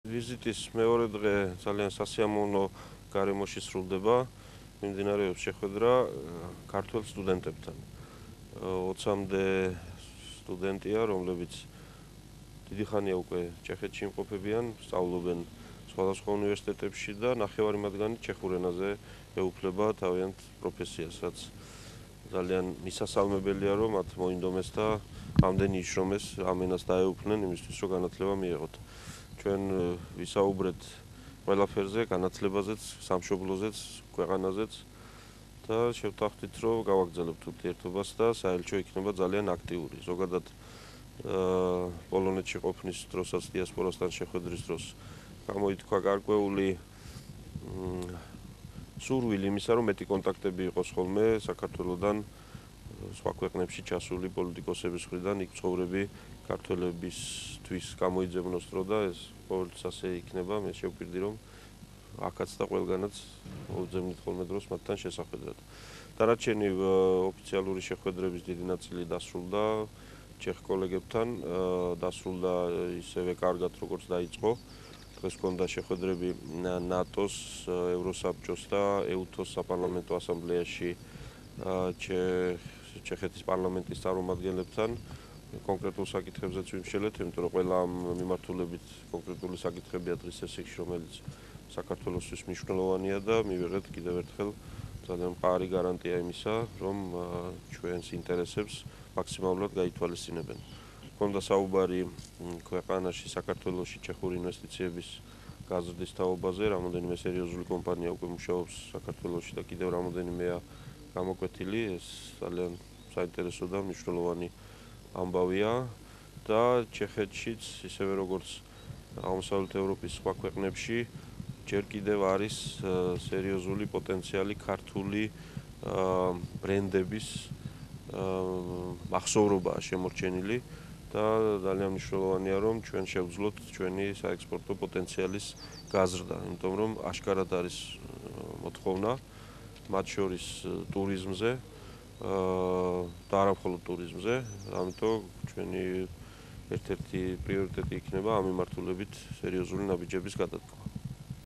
დავით უსუფაშვილი ჩეხეთში ქართველ სტუდენტებს შეხვდა
დავით უსუფაშვილი, საპარლამენტო დელეგაციასთან ერთად ჩეხეთში საქართველოს საელჩოს ეწვია და საელჩოს თანამშრომლებს და ჩეხეთის სხვადასხვა უნივერსიტეტების ბაკალავრიატის, მაგისტრატურისა და დოქტორანტურის ქართველ სტუდენტებს შეხვდა.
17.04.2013